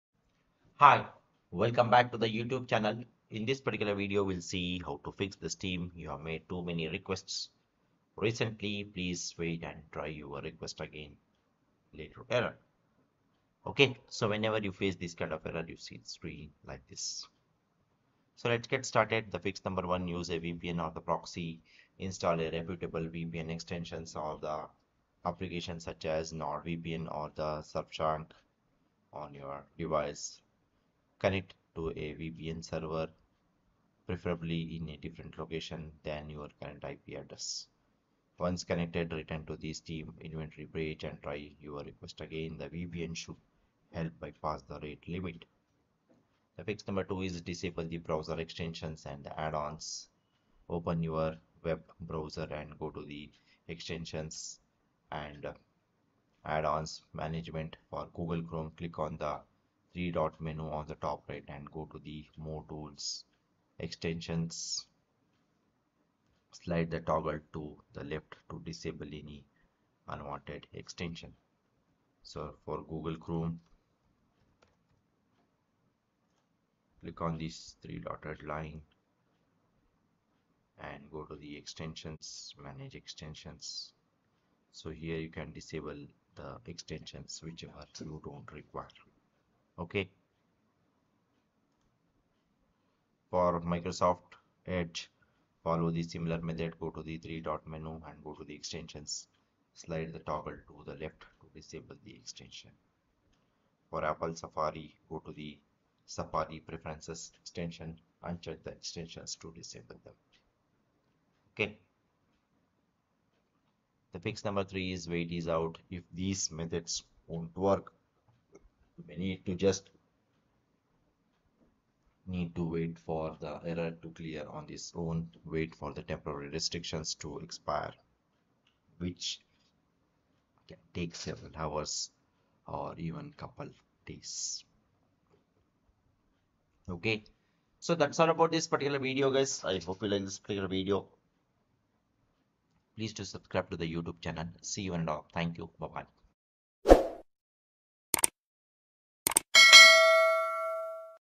To Fix Steam You've Made Too Many Requests Recently Please Wait & Try Your Request Again Later Error
Today, we're going to show you How To Fix Steam You've Made Too Many Requests Recently Please Wait And Try Your Request Again Later Error. This can be helpful in various situations, such as Fixing this error allows uninterrupted access, prevents frustration, improves user experience, ensures account functionality, and maintains smooth interaction with Steam services.

0:21 Fix 1. Use A VPN Or Proxy
0:47 Fix 2. Disable Browser Extensions And Add-Ons
1:55 Fix 3. Wait It Out